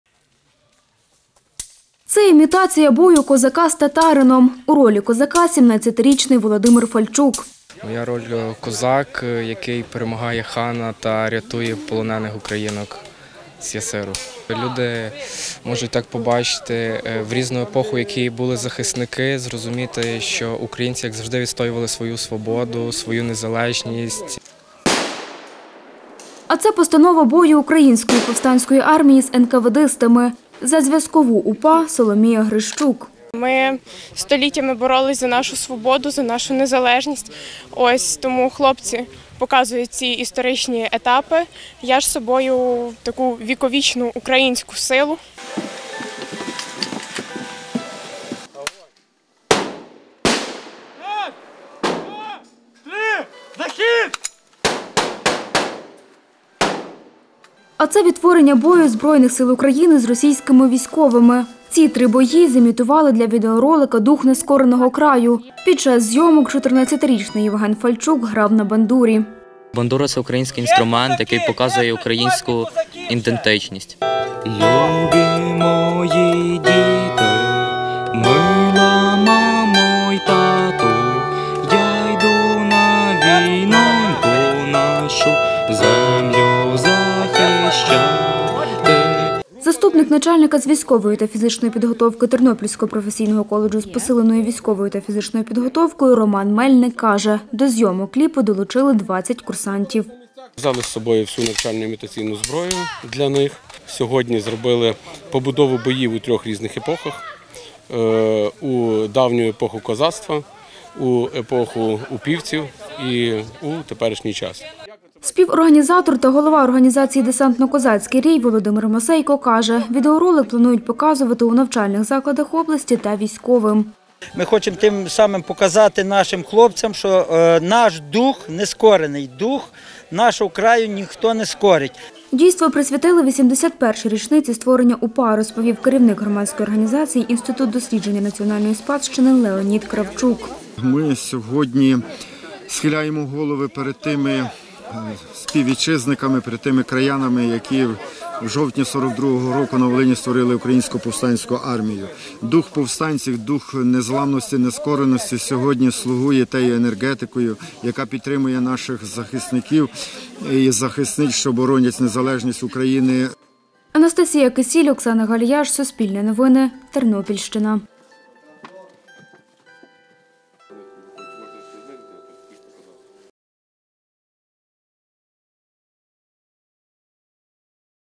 Патріотичний відеоролик знімають на Тернопільщині
На Тернопільщині знімають відеоролик "Дух нескореного краю". У ньому відтворюють три етапи української історії: козацька доба – визволення полонянок з татарської неволі, бій повстанців з енкаведистами та бій ЗСУ з російськими військами. Як це було – у сюжеті.